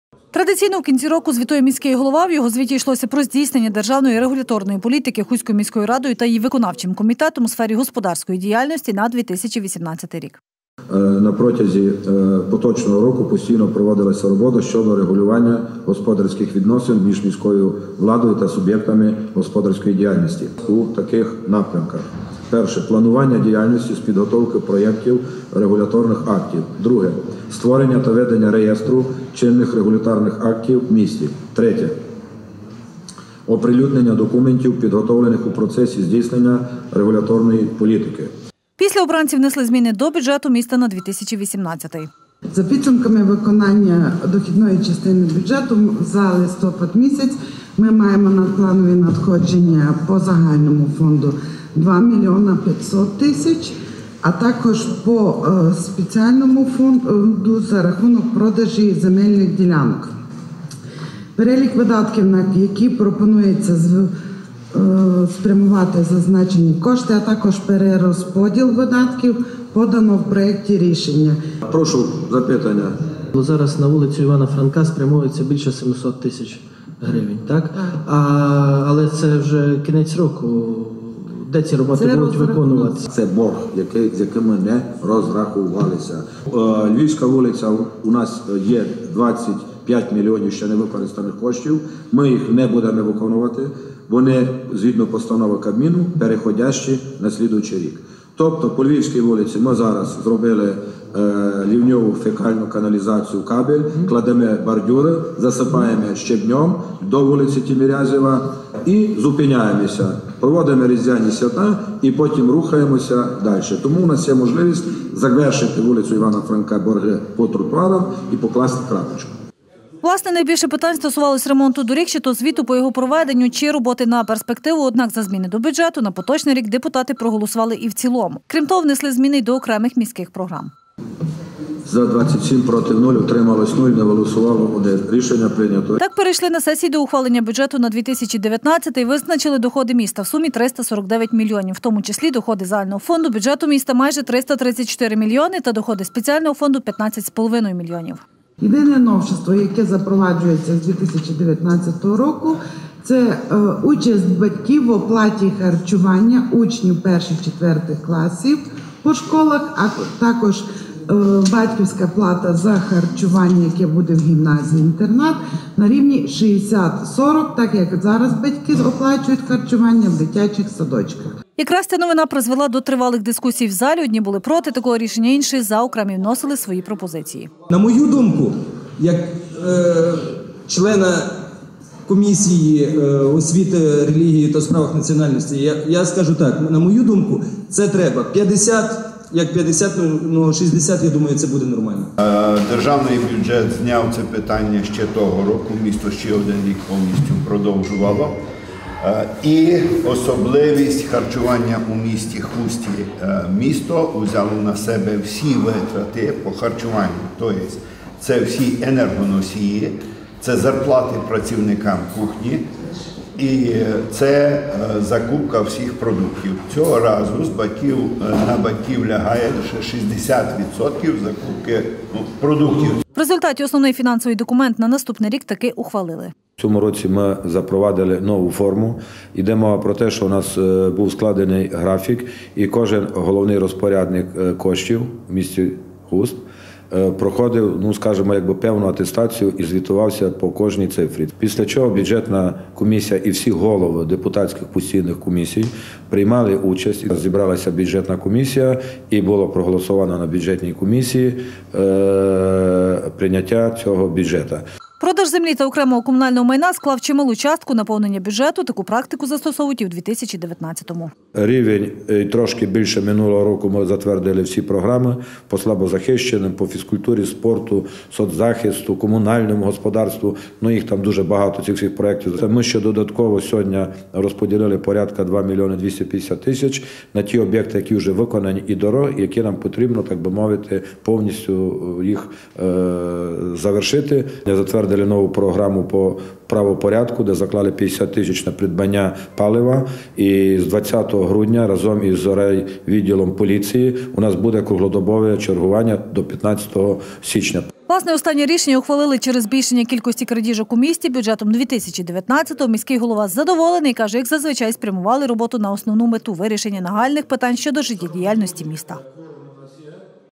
Бюджетна сесія
У Хусті прийняли бюджет на 2019 рік. Загалом на 9-му засіданні 8-ї сесії Хустської міської ради депутати розглянули більше 60-ти питань, одне з яких якраз і стосувалося бюджету міста на рік наступний.